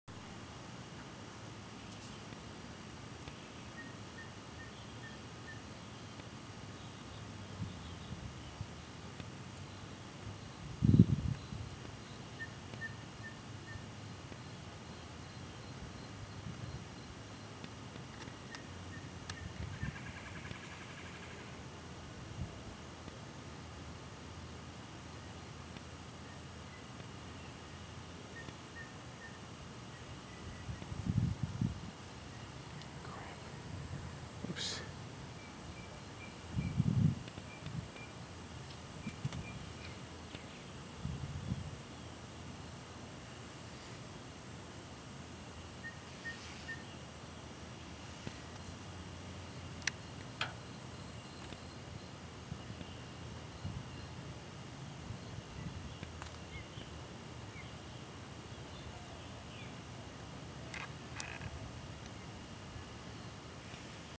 Florida Lizard Encounter #15
This video shows of Florida's wild lizards. This was filmed on June 2014.